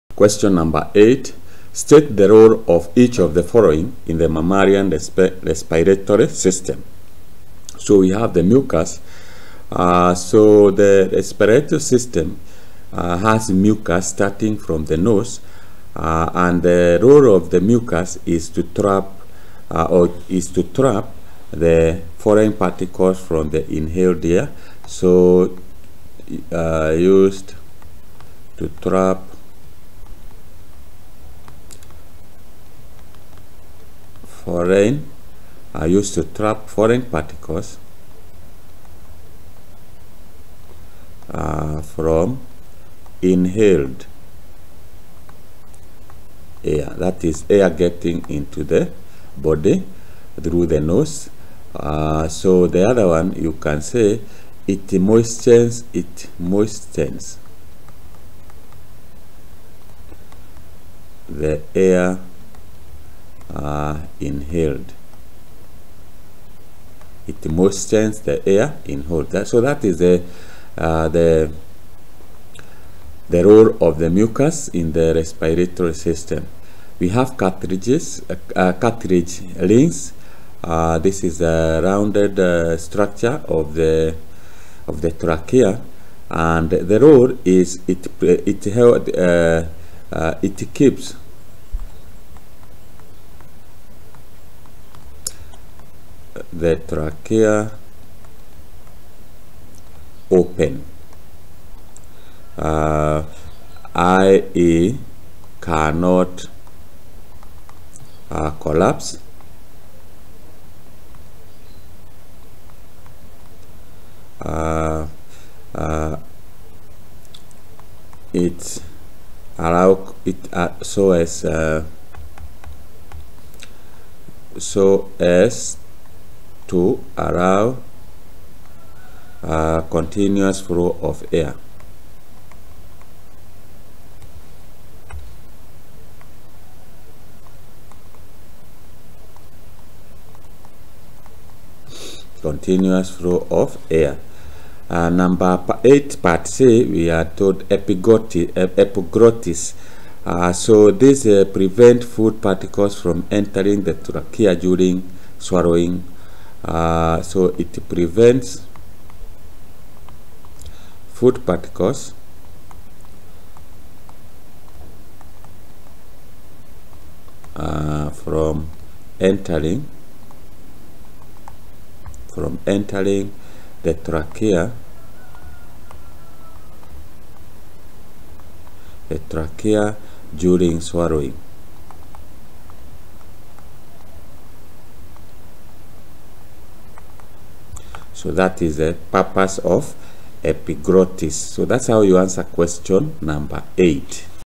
2019 KCSE Biology Paper 1 Question 8
This video explains how to correctly answer Question #8 on KNEC's 2019 KCSE Biology Paper 1.

g. State the role of each of the following in the mammalian respiratory system:

(a) mucus (2 marks)

(b) cartilage rings (1 mark)